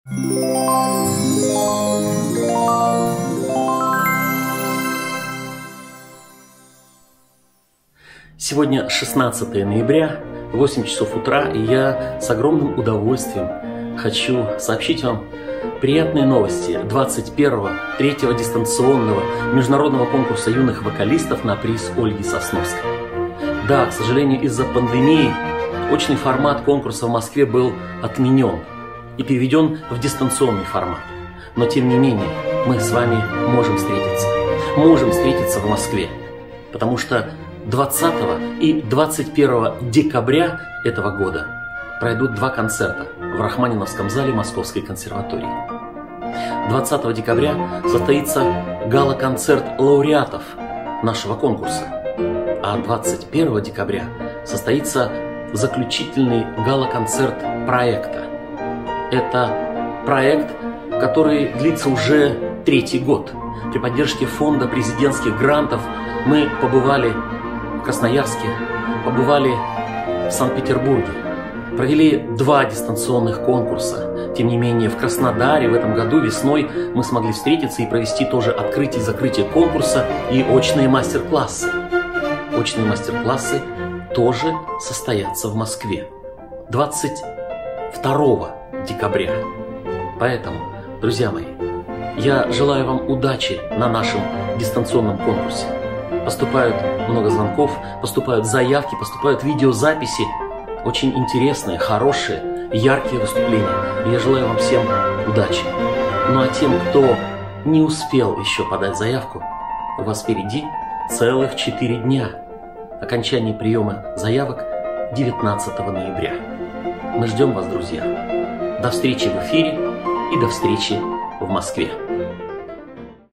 Владимир Юрковский об очной части программы III-го дистанционного конкурса
III-й дистанционный | Организатор конкурса Владимир Юрковский анонсировал два «живых» Гала-концерта в Московской консерватории

– Сегодня 16 ноября, и я с огромным удовольствием хочу сообщить вам приятные новости XXI (III-го дистанционного) Международного конкурса юных вокалистов на приз Ольги Сосновской.

Да, к сожалению, из-за пандемии очный формат конкурса в Москве был отменен и переведен в дистанционный формат. Но, тем не менее, мы с вами можем встретиться в Москве, потому что 20 и 21 декабря этого года пройдут два концерта в Рахманиновском зале Московской консерватории. 20 декабря состоится Гала-концерт лауреатов нашего конкурса, а 21 декабря – заключительный Гала-концерт проекта, проекта, который длится уже третий год. При поддержке Фонда президентских грантов мы побывали в Красноярске, Санкт-Петербурге, провели два дистанционных конкурса. В Краснодаре этой весной мы смогли встретиться и провести тоже открытие и закрытие конкурса и очные мастер-классы. Очные мастер-классы состоятся и в Москве 22 декабря.

Поступают много звонков, поступают заявки, видеозаписи – очень интересные, хорошие и яркие выступления. И я желаю вам всем удачи! Ну а тем, кто не успел еще подать заявку, у вас впереди целых четыре дня. Окончание приема заявок 19 ноября. Мы ждем вас друзья, до встречи в эфире и до встречи в Москве!